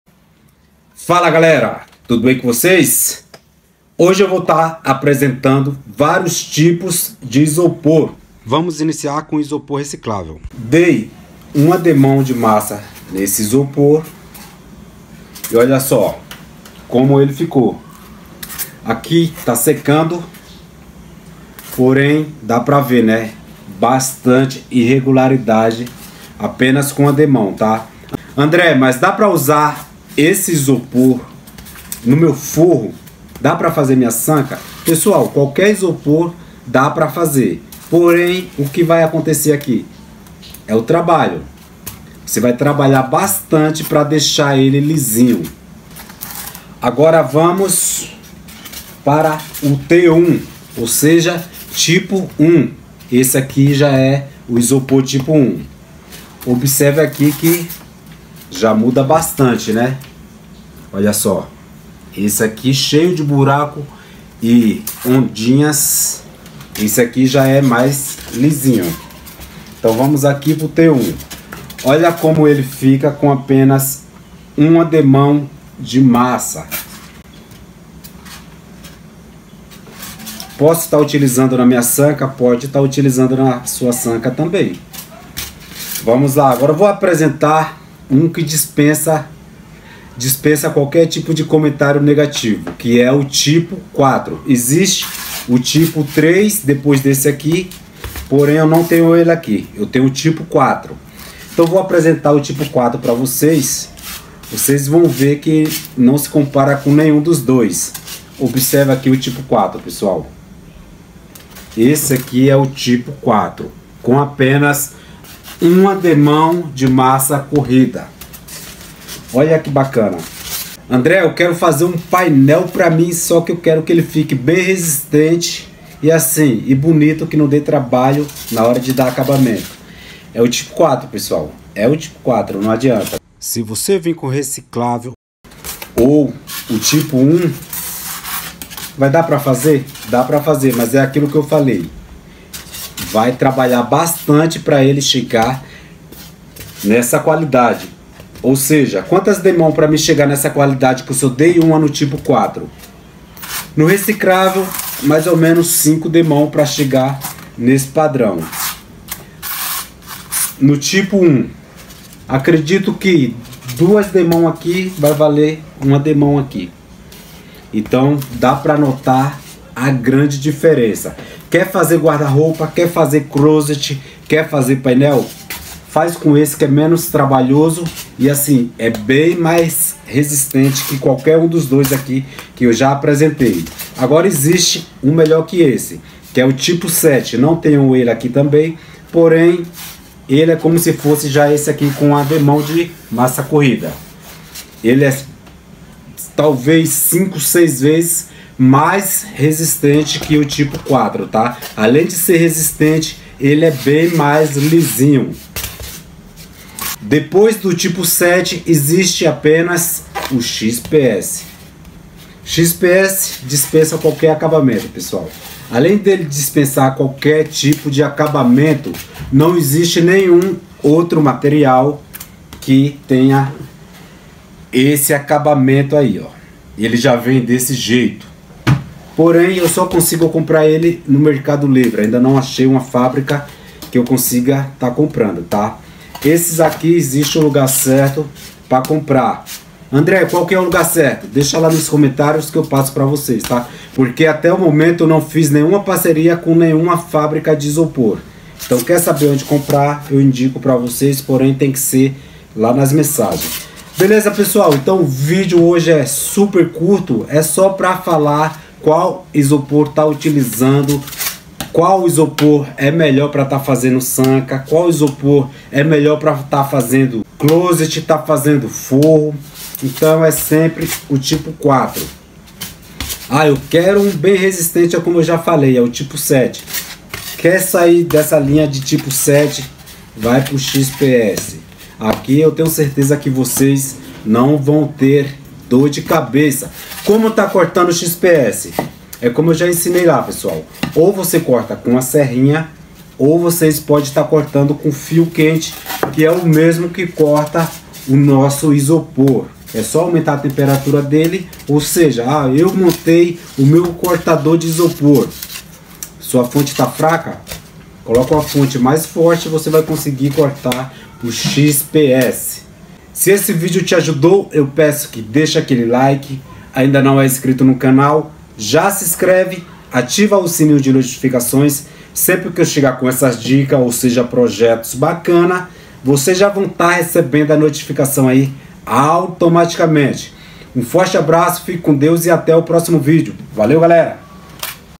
VC JÁ SABE QUAL ISOPOR USAR EM SEUS PROJETOS?
Use o isopor certe e trabalhe menos!

Meu Instagram ARTE_COM_ISOPOR